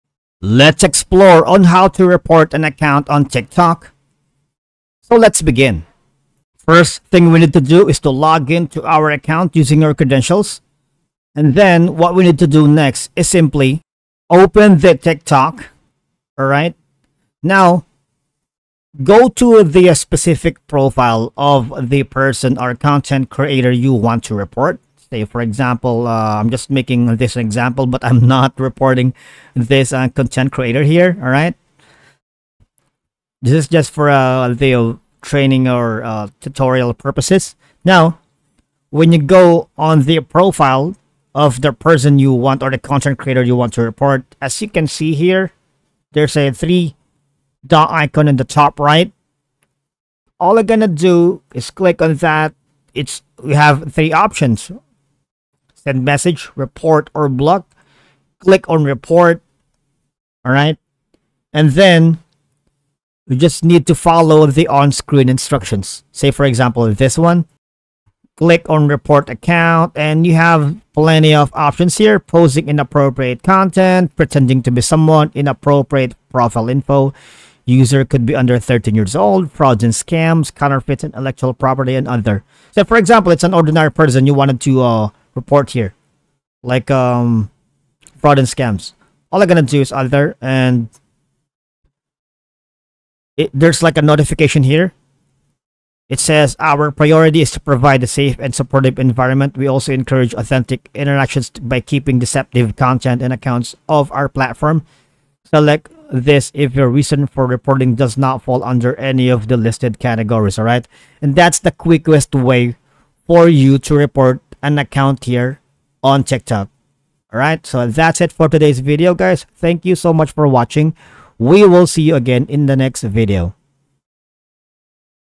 How to Report Account on TikTok (Easiest Way)
In this video, I'll show you How to Report Account on TikTok (Easiest Way). The method is straightforward and clearly described in the video. Follow all the steps & How to Report Account on TikTok (Easiest Way)